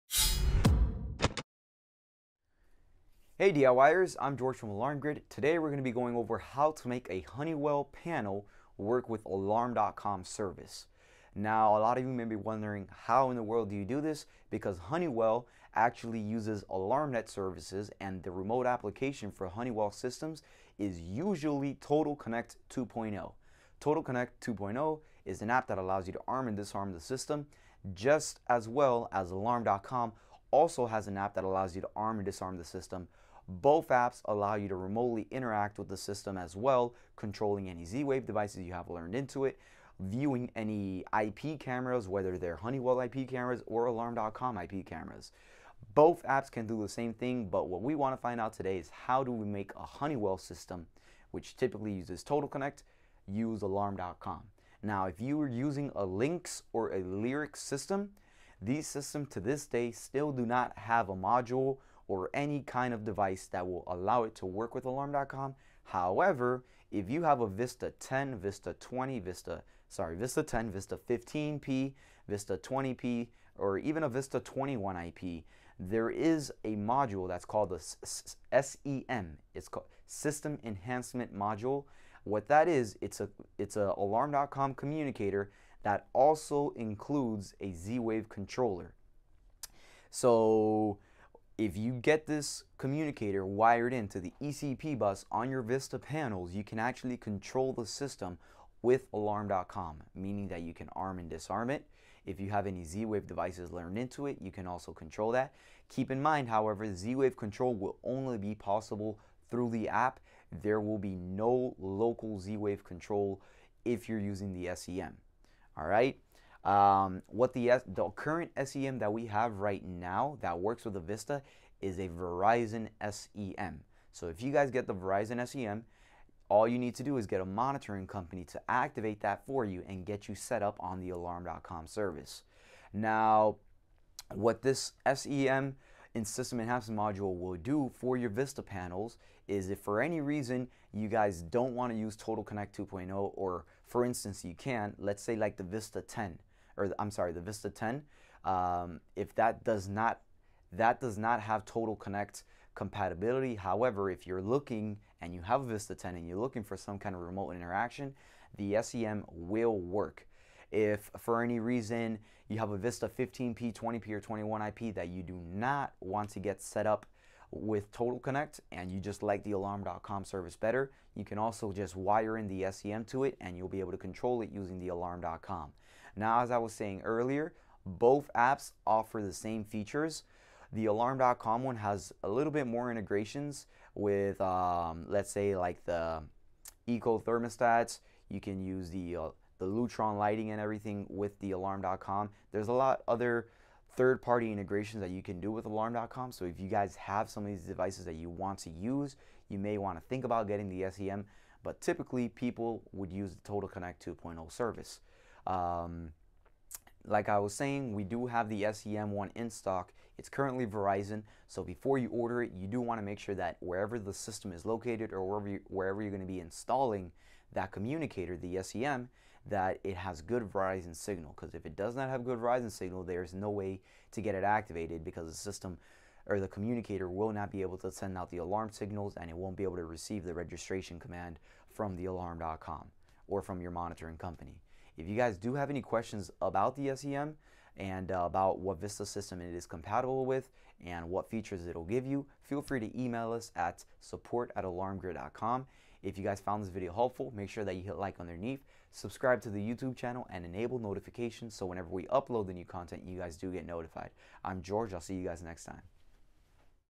Making a Honeywell Panel Work With Alarm.com
In this video, Jorge talks about how some Honeywell Panels can connect with the Alarm.com Service. Normally, a Honeywell Alarm Panel would use the Total Connect 2.0 platform. But the VISTA Panels support various System Enhancement Modules (SEMs) that allow them to connect with the Alarm.com service.

Total Connect 2.0 and Alarm.com are two similar interactive service platforms that allow a user to control their security system remotely. They are also used for integrating certain smart home devices into one convenient location. In most cases, a user will be largely indifferent as to whether they use TC2 or ADC, as they mostly do the exact same thing.

However, there are certain cases where a user might prefer Alarm.com. For example, Alarm.com is compatible with their own lineup of security cameras that will not work with Total Connect 2.0. Alarm.com is also compatible with Google Home, while Total Connect 2.0 is not. These are some reasons why a user might choose to get an SEM instead of a conventional communicator.

By using an SEM, a VISTA Panel will communicate with the Alarm.com Servers across a cellular network. This is important, as cellular communication is extremely reliable and unaffected by power outages. This way, a user can have peace of mind in knowing that their signals will go through successfully. Cellular communication for alarm systems is also very fast, and a user will receive a quick response in the event of an alarm.

An SEM also provides Z-Wave control for a VISTA Panel. But by using an SEM, all Z-Wave control will need to take place through Alarm.com. This can be done on the Alarm.com website or by using the Alarm.com Mobile App on an Android or iOS device. There will be no way to control Z-Wave devices locally from the panel.